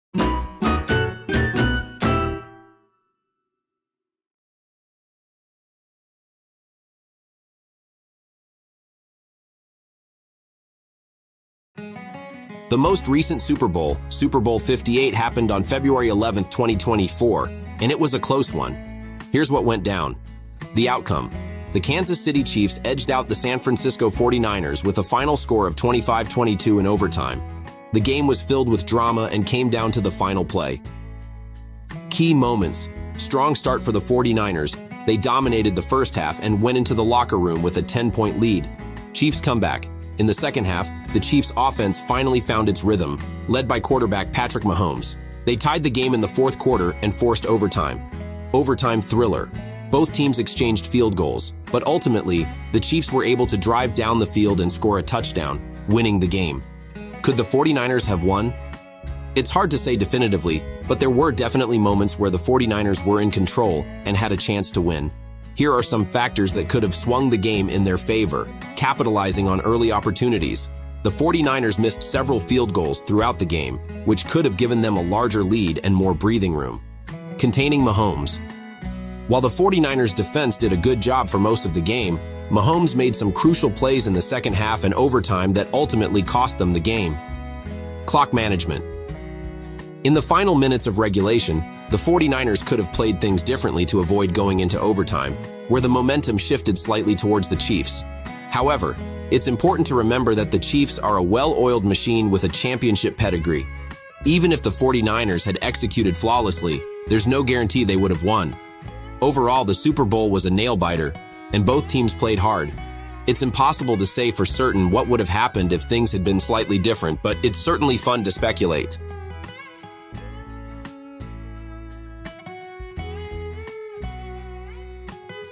Can the 49ers Win Super Bowl LVIII 2024? I Asked GEMINI AI for the Prediction!
Did the 49ers have a WINNING CHANCE at Super Bowl LVIII? AI analyzes key plays & reveals what COULD have changed the game! Inside insights and breakdown! Don't miss this SuperBowl analysis!